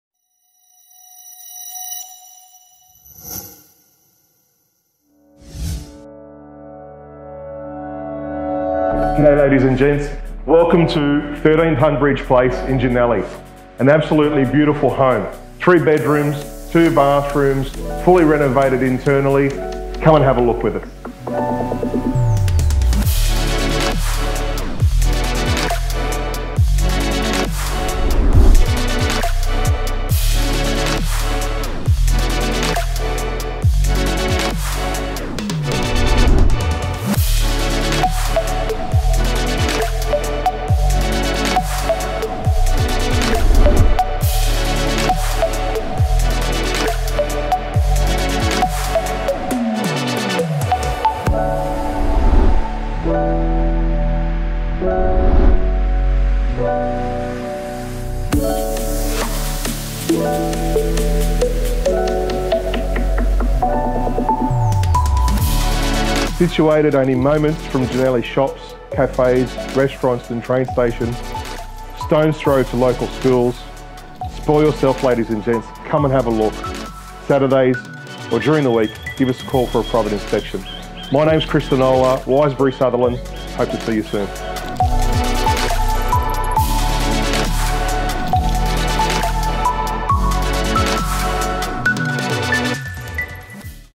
13 Tunbridge Place Jannali
Ideal Family Home.
This spacious family residence provides comfortable living across a thoughtful open plan design. Upon inspection you will be surprised with the level of quality craftsmanship that has been carried out to offer a truly unique home. The ground floor delivers 2 large bedrooms with built-ins, a recently renovated bathroom, a study/kickout area, ample storage and high ceilings that carry on throughout the home.
Moving on upstairs to the open plan living area is complete with a feeling of space, light and quality, a large bespoke kitchen, including an induction cooktop, that overlooks the dining and living area that brings together the perfect entertainer’s home.
This main level also offers the master bedrooms with built ins and a recently renovated generous ensuite. Finishing of the main living area is a large, renovated laundry with 3rd WC.

You’ll have easy access to well respected local schools and only 5 minutes’ drive to cafes, restaurants, and shopping precinct of Jannali.
All this nestled on a 642.5 sqm of land, this home will appeal to a range of purchasers, so give me a call today for a private inspection.

• 3 Bedrooms main with newly renovated ensuite.
• Generous newly renovated main bathroom.
• Large open plan living areas with custom cabinetry and plantation shutters
• Well-appointed kitchen with induction cooktop, soft close draws and     plenty of storage.
• Newly renovated Laundry and 3rd WC
• Covered outdoor entertaining area
• Covered off-street parking and under house storage.